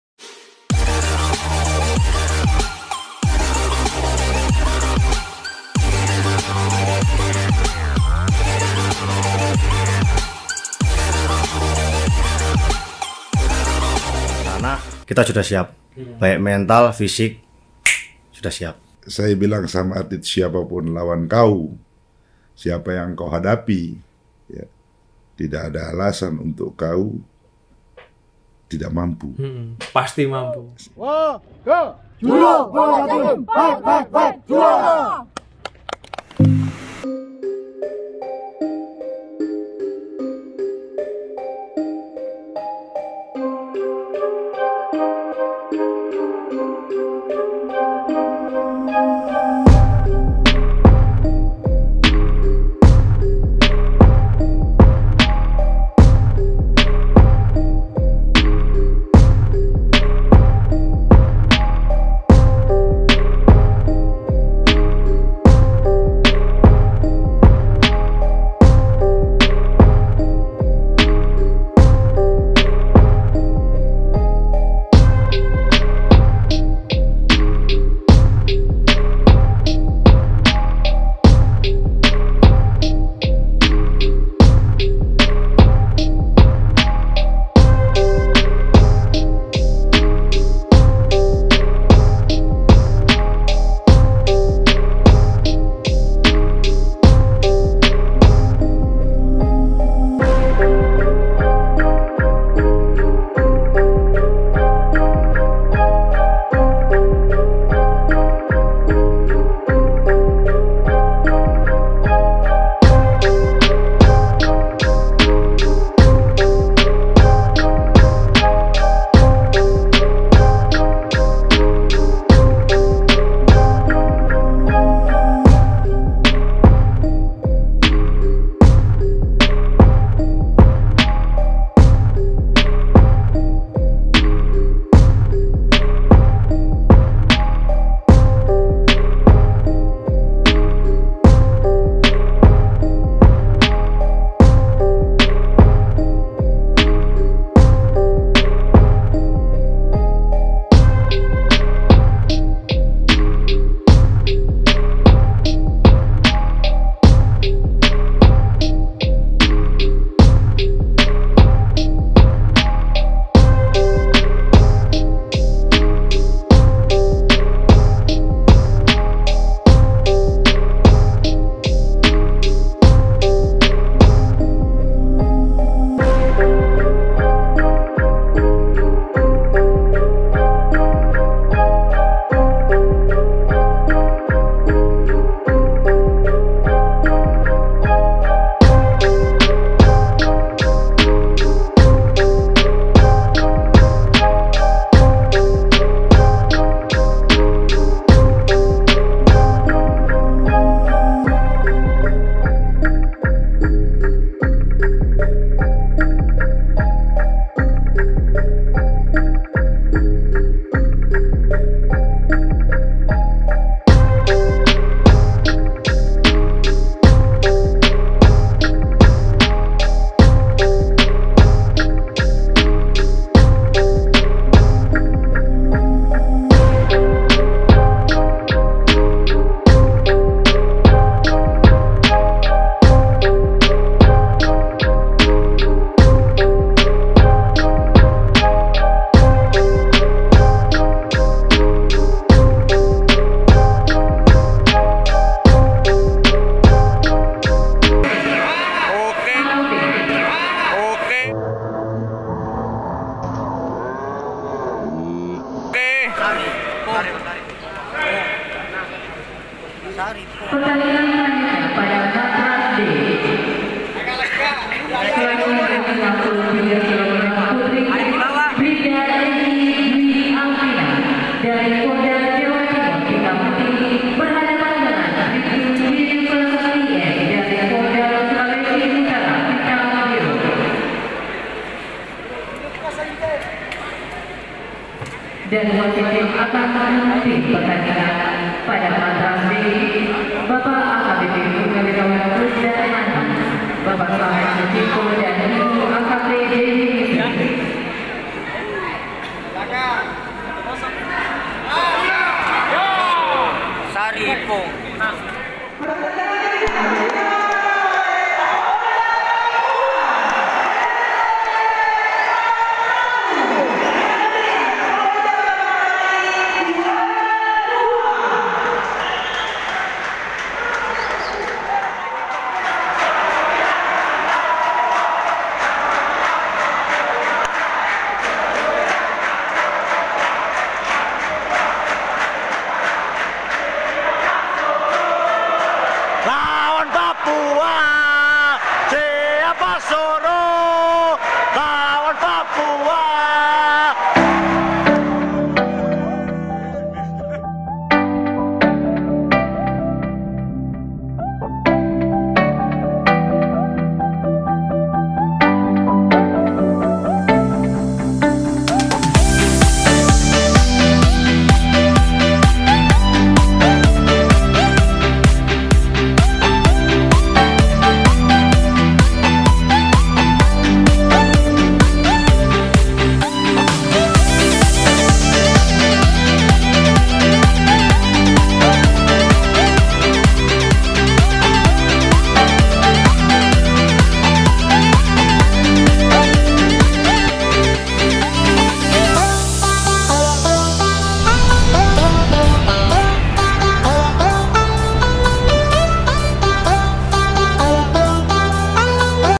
Atlet Polda Jatim ini Kejuaraan Kapolri Cup 2022 tidak diragukan lagi rebut Juara Umum || Sat Brimob
Atlet Judo Polda Jatim dalam rangka Kejuaraan Kapolri Cup 2022 yang diselenggarakan di Polda Sultra berjalan lancar dan terkendali. Dan bisa merebut juara umum 2 .

Sumber video : Bid Humas Polda Jatim